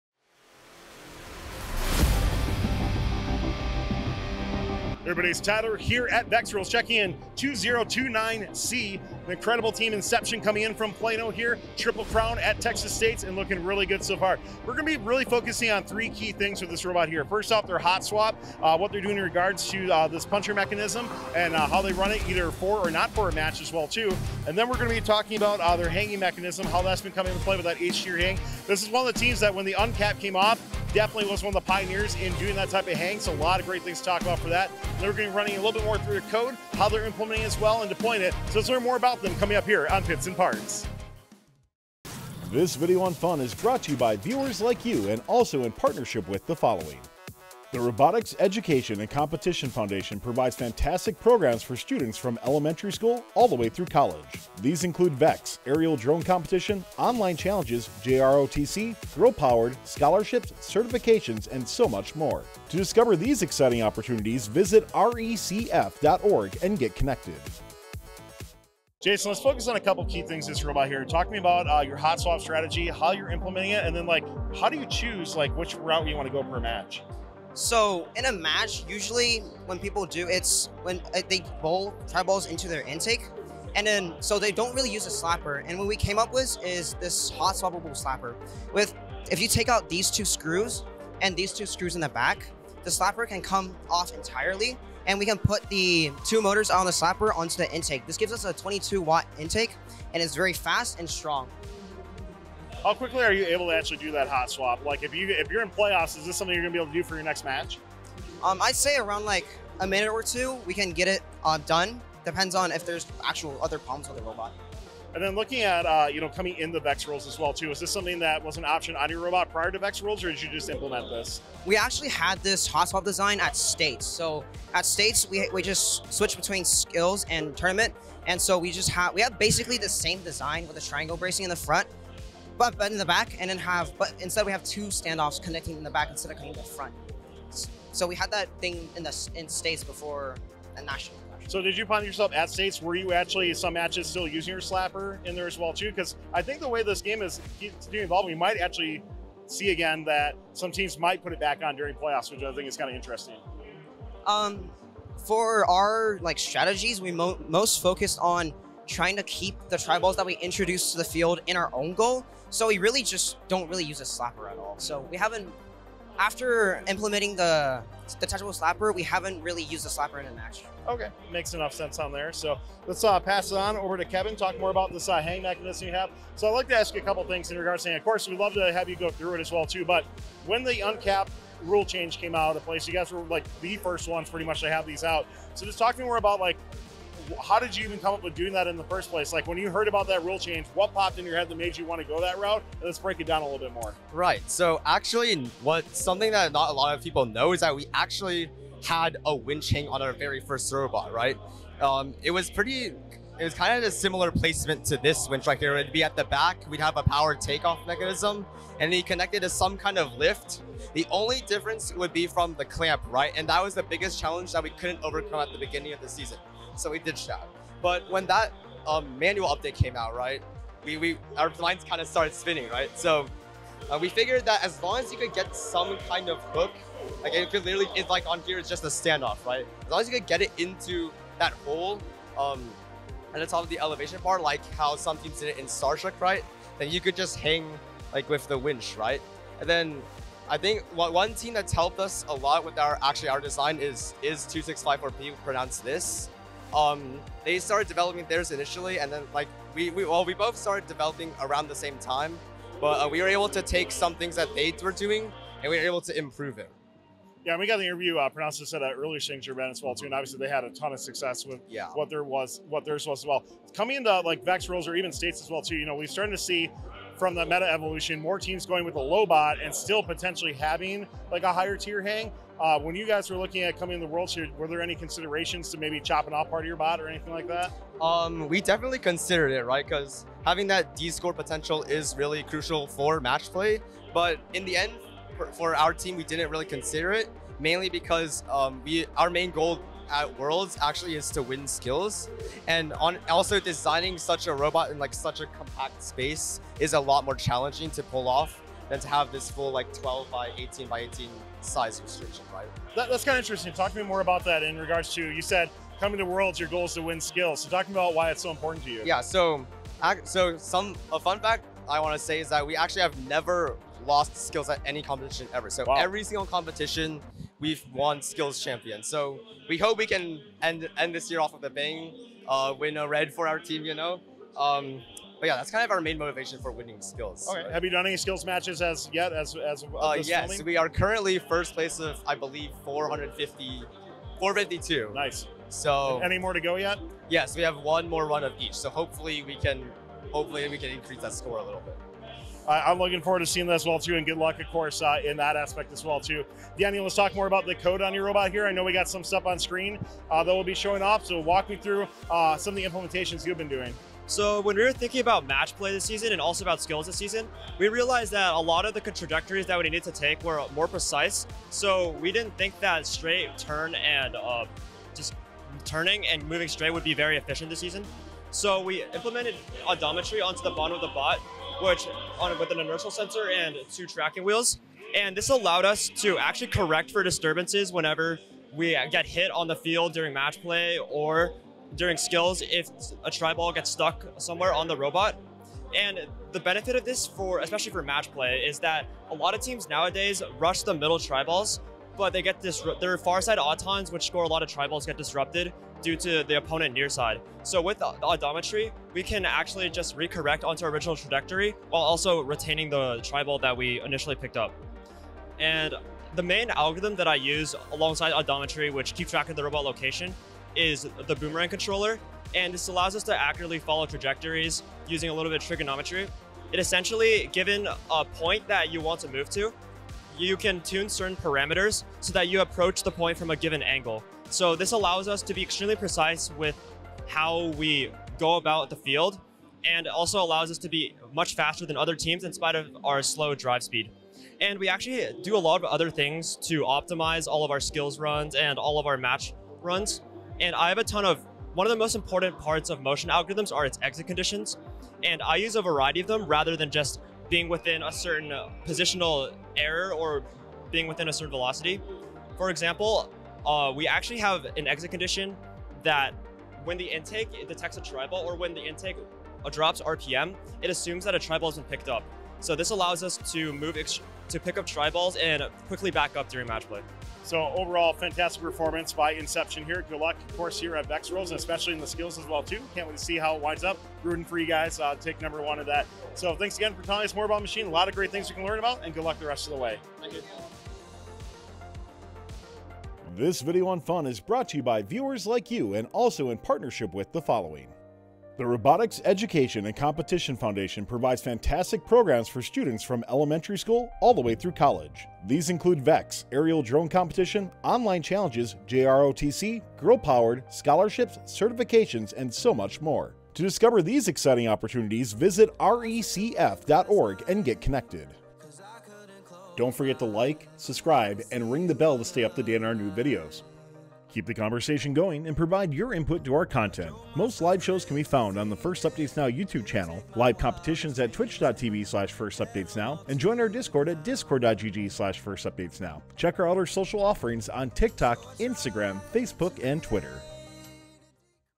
2029C Inception | World Skills Champion | Pits & Parts | Over Under Robot
2029C Inception had an insane amount of qualifying awards capping it off as your Over Under Skills Champion at Worlds. 2029C focuses on their H-tier hang and discusses being one of the first teams to go that route, their code and hot-swap punching mechanism on Pits & Parts.